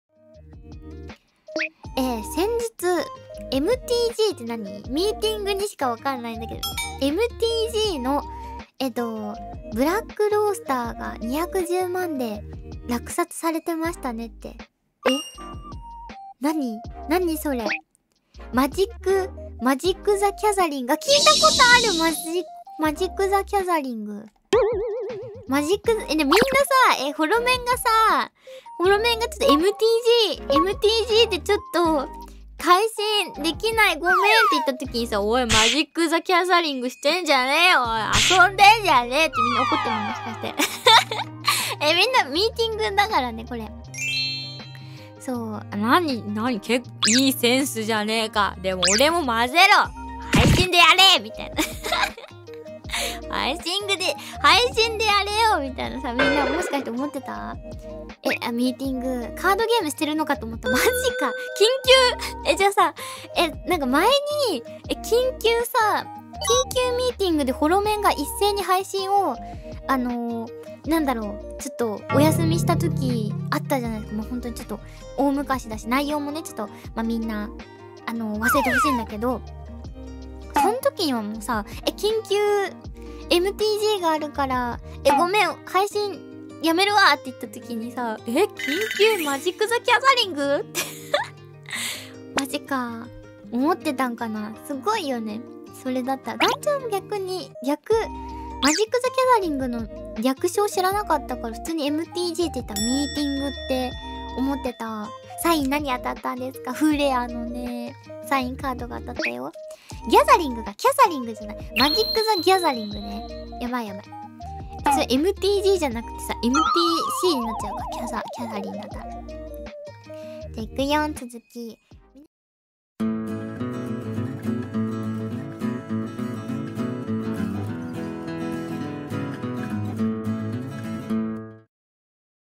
【白銀ノエル】MTGが何の略か分からないノエル【ホロライブ切り抜き】
2021/10/06の配信「【開封カメラ枠】ヴァイスシュヴァルツ×ホロライブ！サインカードを当てたい！【白銀ノエルホロライブ】  」からの切り抜きです。